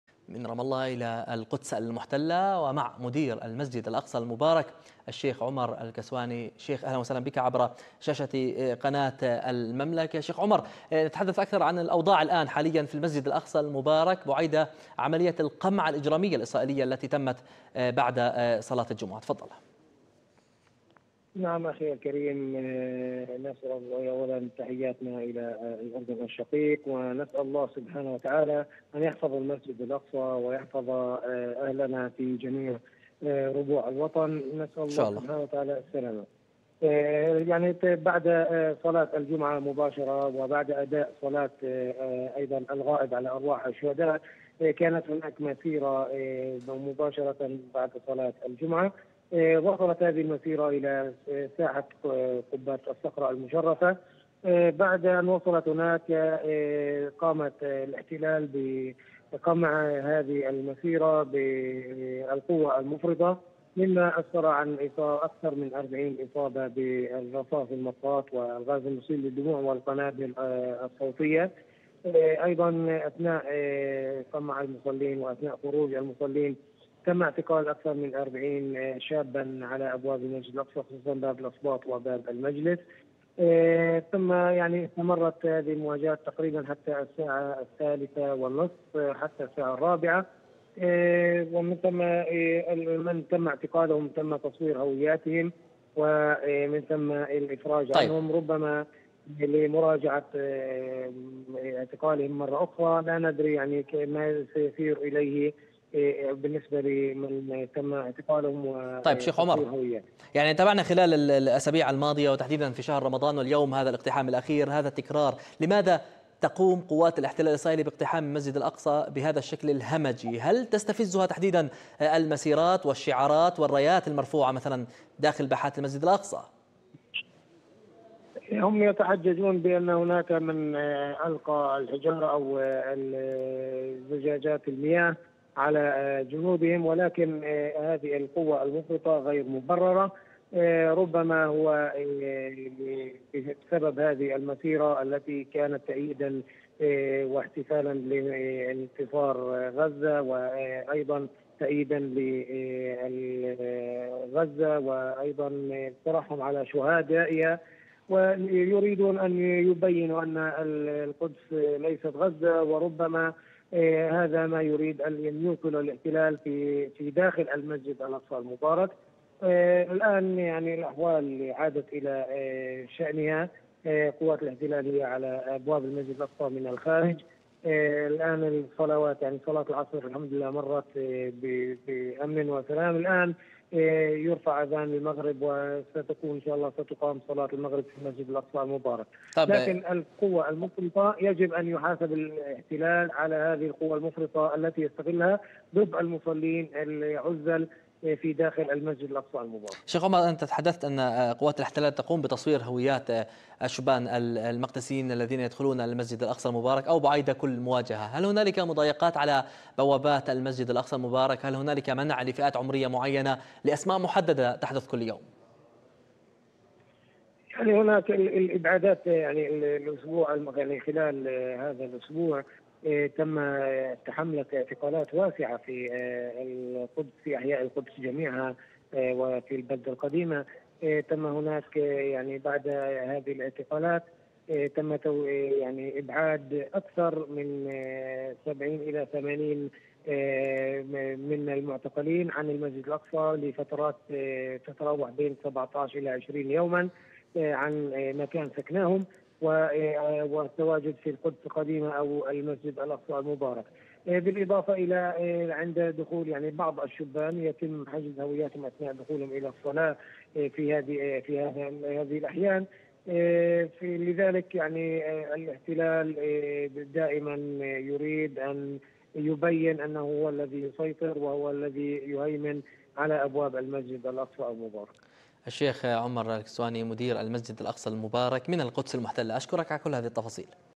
قوات الاحتلال الإسرائيلي تعتدي على المصلين في المسجد الأقصى
مدير المسجد الأقصى المبارك الشيخ عمر الكسواني